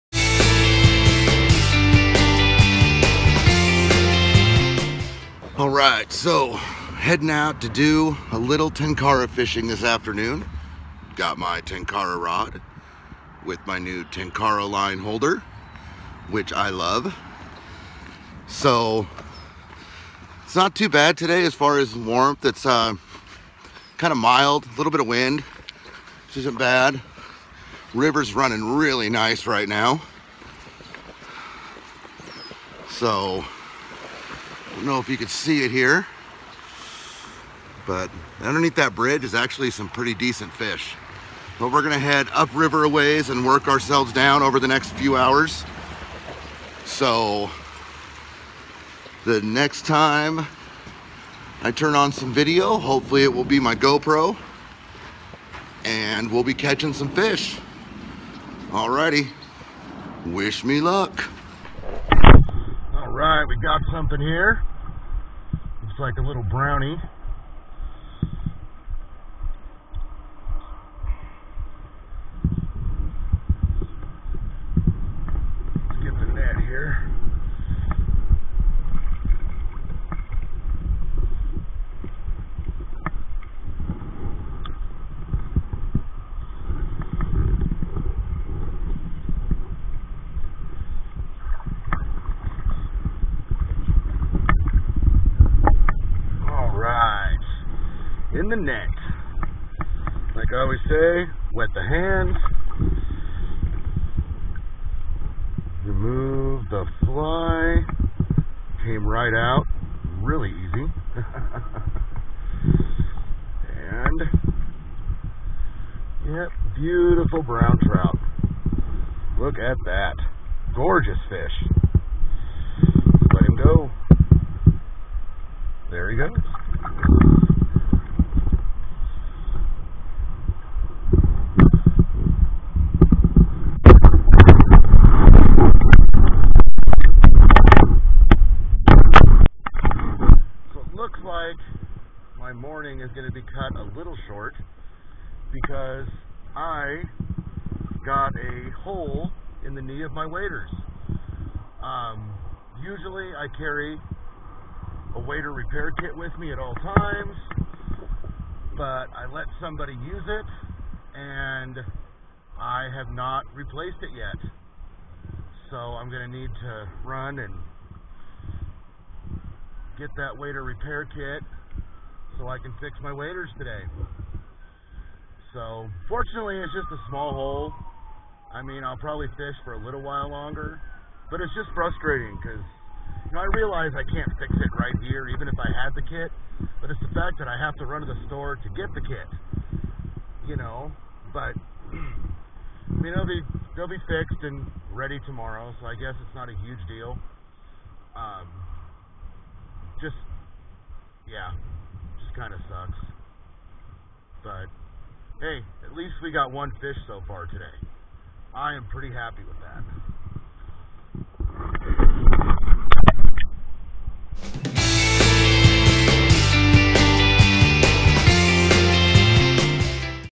Tenkara 1 Fish And A Hole In My Waders
Day cut short but at least I caught one fish